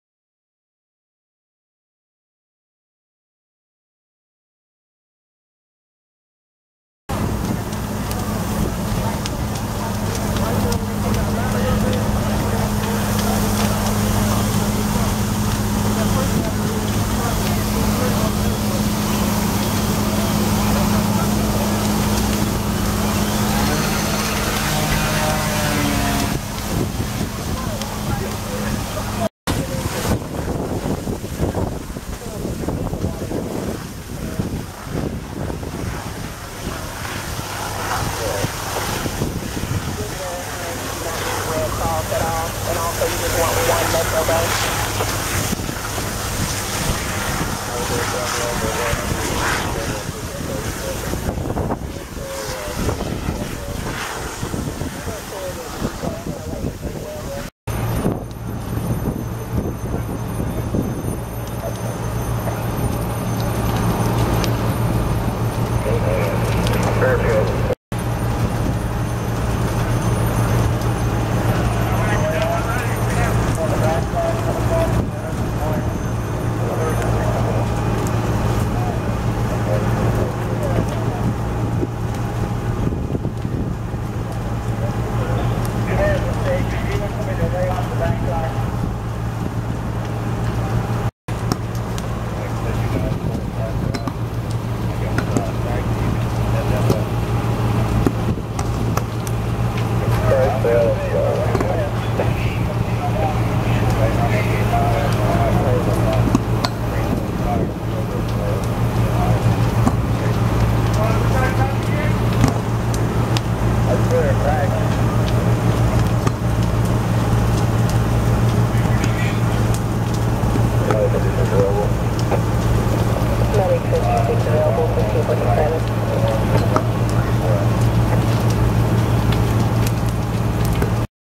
2/18/2015 Colerain Twp, OH 4 Alarm Structure Fire on Walden Glen Cir Ashley Woods Apartments
Multiple fire departments responded to the Ashley Woods Apts for the report of an apartment on fire. They found a kitchen fire which extended into the walls. Strong winds and problems with the private hydrants blew the fire along the roof line until the fire wall stopped it. After a total of 4 alarms were transmitted, crews were able to stop the fire but all 8 apartments were lost. There were no injuries to any occupants.